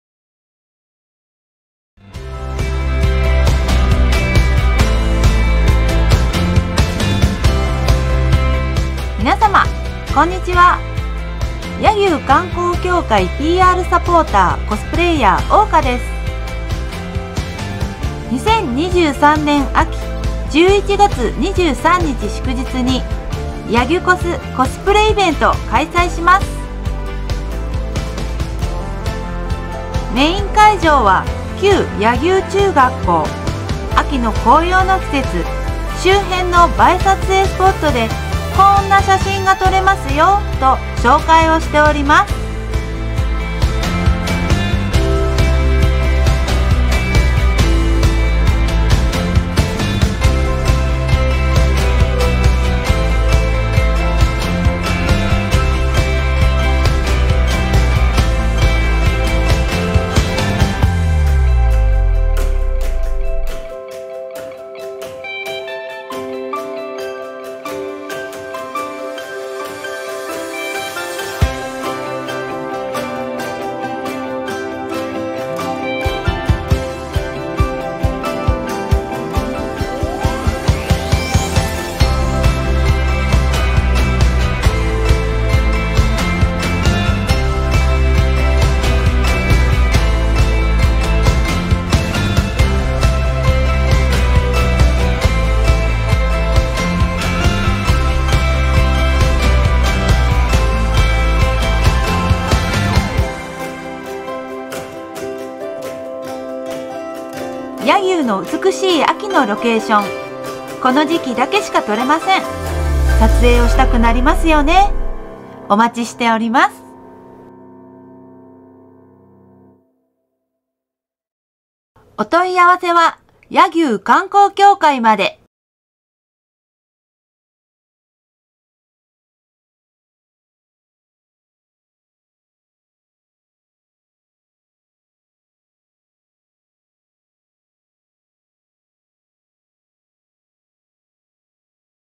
第８回 やぎゅこすコスプレ撮影会 剣聖の里 柳生 2023.11.23
奈良県奈良市柳生町。剣聖の里柳生はコスプレイヤーさんの聖地です。
町全体がいわば自然のスタジオ。沢山のコスプレイヤーさんが来訪し、思い思いのコスチュームで、こだわりの写真撮影などを楽しんでいます。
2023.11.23にはやぎゅこすコスプレ撮影会が開催されます。詳しくは、柳生観光協会のホームページをご覧ください。